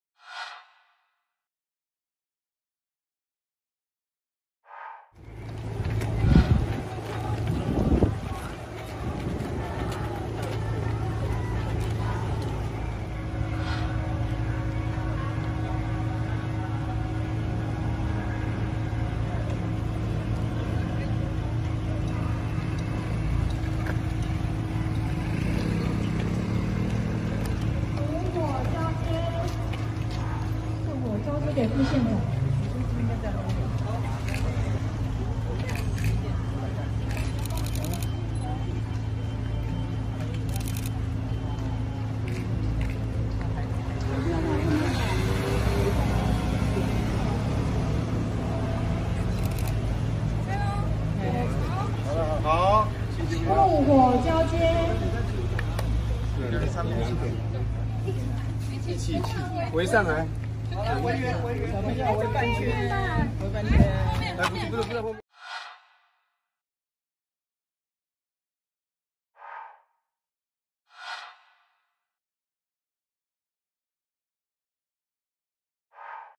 馬祖日報2020-09-02影音／3進3出台北完傳 第8屆離島運聖火馬祖傳遞金門
日期：2020/09/02日下午14:00
地點：金門縣政府、金寧鎮
資料：連江縣政府教育處
　象徵離島3縣市跨島合作交流，第8屆離島運聖火最後1站傳遞金門，在金門縣政府前舉行聖火傳遞交接儀式，大會總幹事陳冠人處長將聖火交由金門副縣長黃怡凱接下火炬後，再引燃副火傳給金門縣代表隊展開傳遞。
　馬祖聖火傳遞小組1日至3日扮演「空中飛人」，1日南竿飛台北，再由台北飛澎湖，2日從澎湖飛台北，由台北轉機到金門，3日由金門飛台北，下午將由台北返回馬祖，3進3出台北，完成聖火傳遞神聖任務。